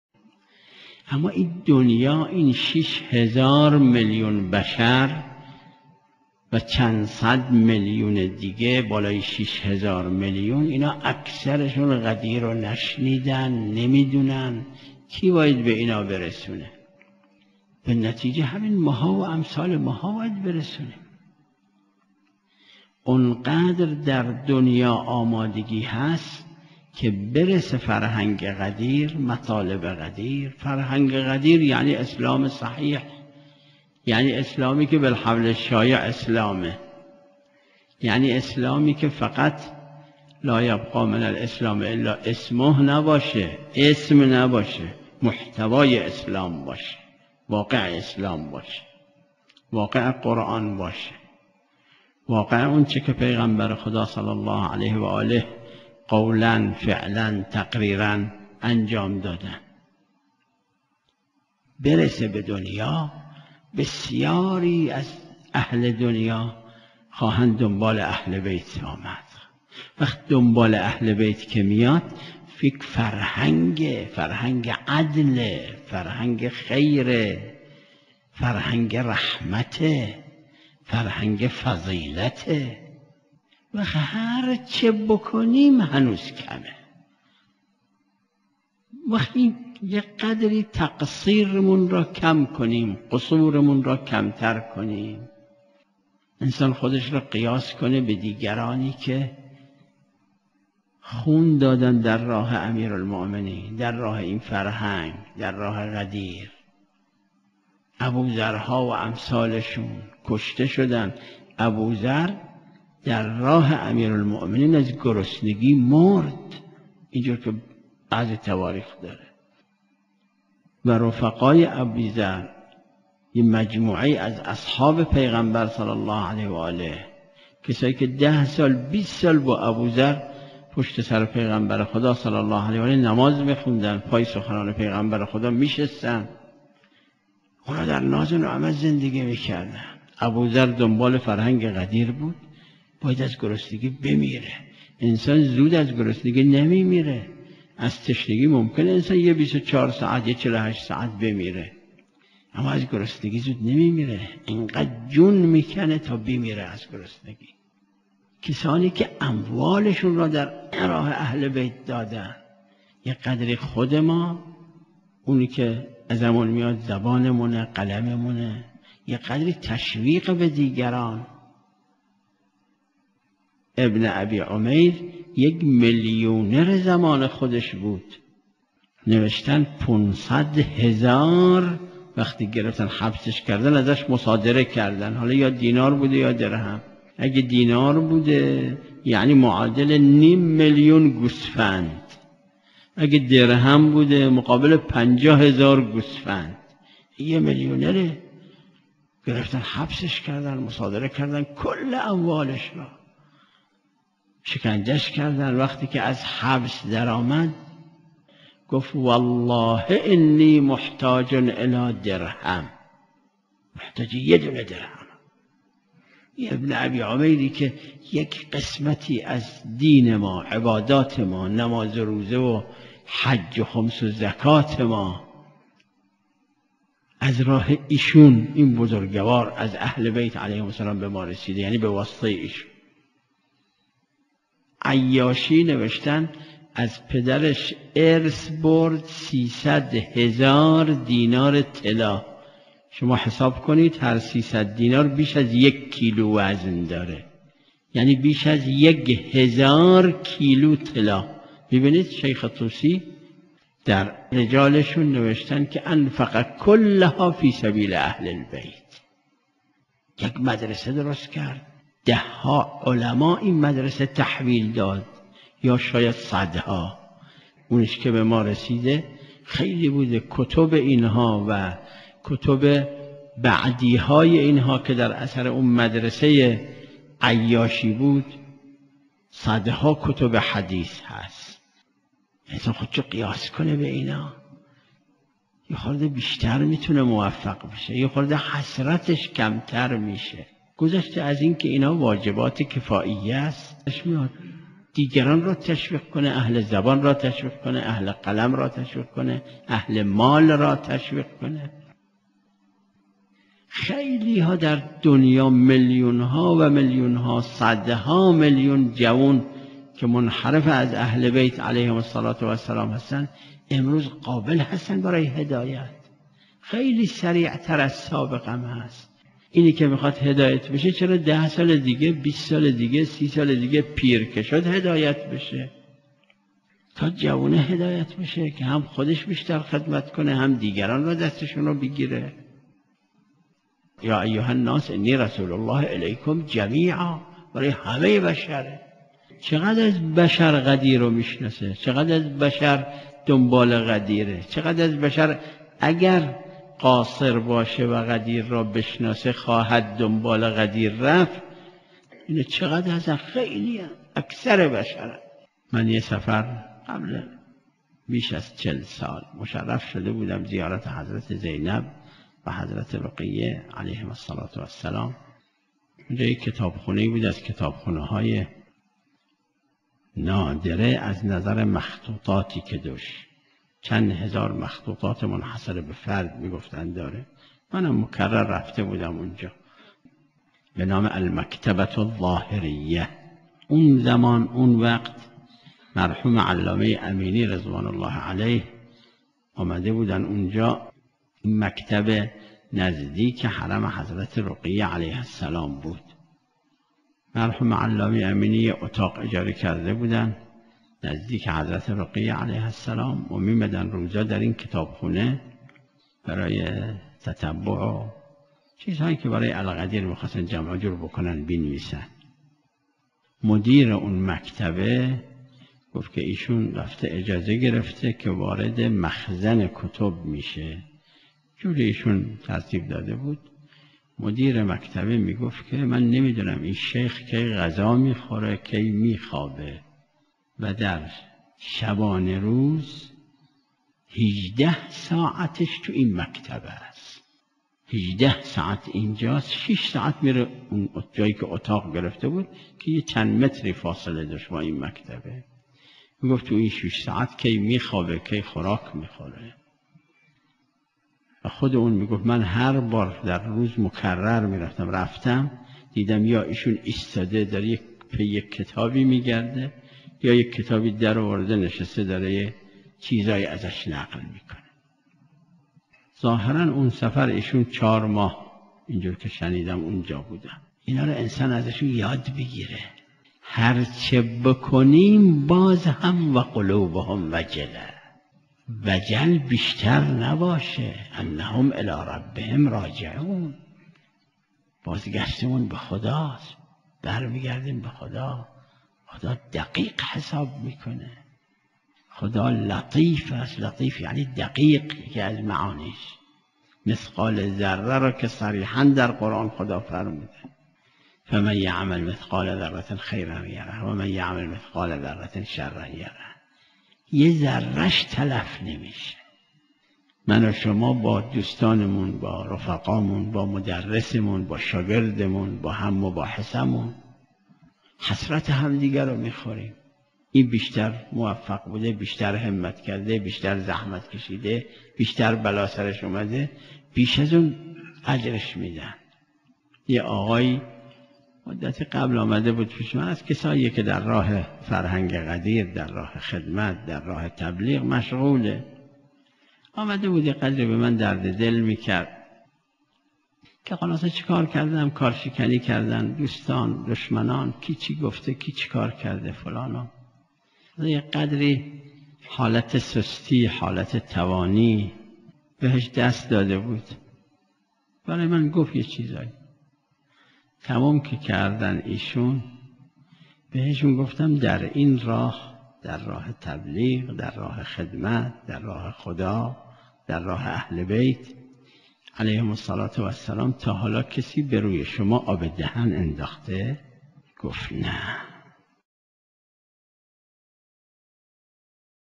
The Culture Of Ghadir by Ayatollah Shirazi (2/3)
A lecture about the culture of Ghadir by
Ayatollah Seyed Sadiq Shirazi.
Lang: Farsi with English Subs.